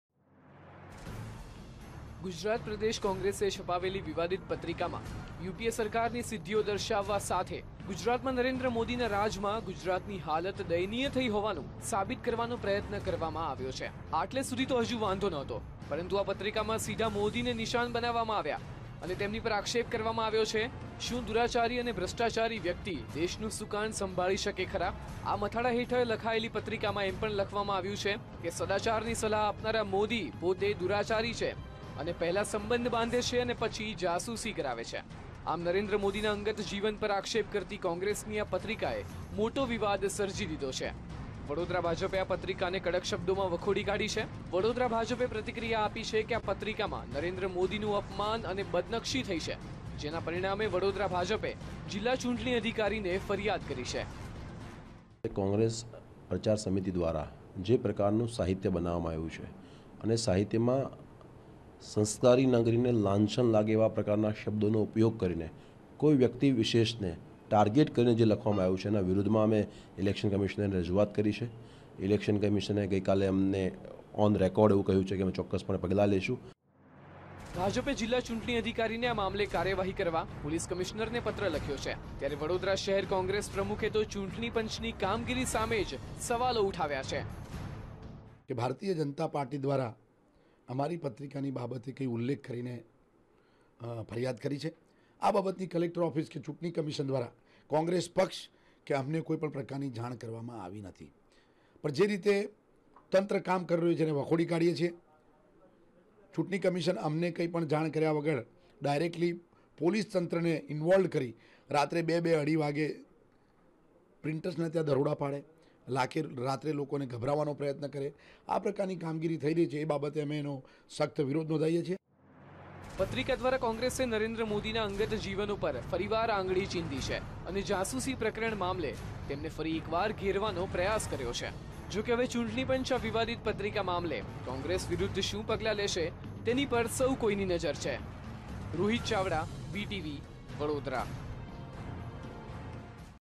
CONTROVERSY ON LEAFLET PRINTED BY CONGRESS, BARODA - VTV
ગુજરાત પ્રદેશ કોંગ્રેસે છપાવેલી પત્રિકાએ વિવાદનું સ્વરૂપ ધારણ કર્યું છે. કોંગ્રેસે છપાવેલી પત્રિકામાં ભાજપના ઉમેદવાર નરેન્દ્ર મોદીની બદનક્ષી થતી હોવાનો આક્ષેપ ભાજપે કર્યો છે. કોંગ્રેસની આ પત્રિકા મામલે વડોદરા ભાજપે વડોદરા જીલ્લા ચૂંટણી અધિકારીને ફરિયાદ કરી છે. તો કોંગ્રેસે તો ચૂંટણીપંચની કામગીરી સામે જ સવાલો ઉઠાવ્યા છે.